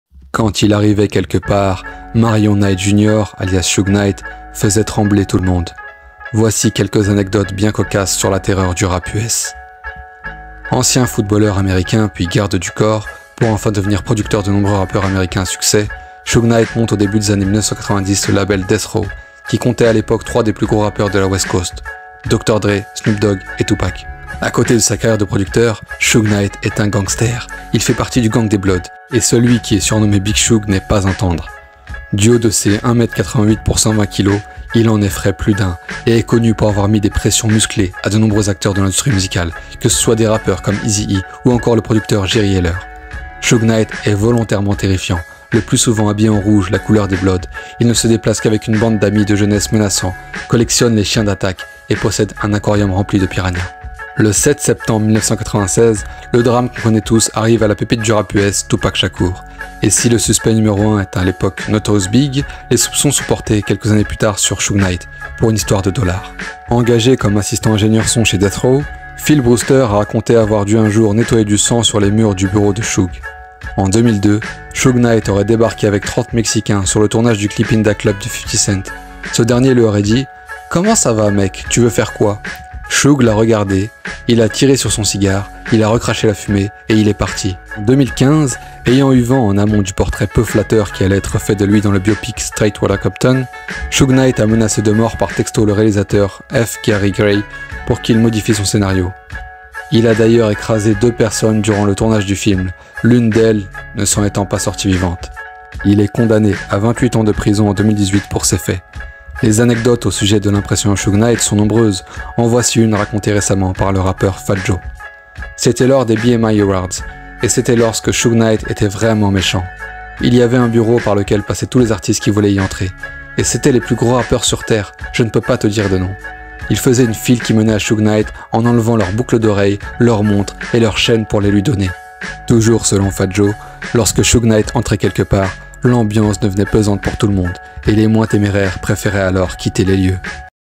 Suge Knight, Terreur du Rap US
Quand il arrivait quelque part, les rappeurs prenaient leurs jambes à leur cous... Quelques anecdotes sur Suge Knight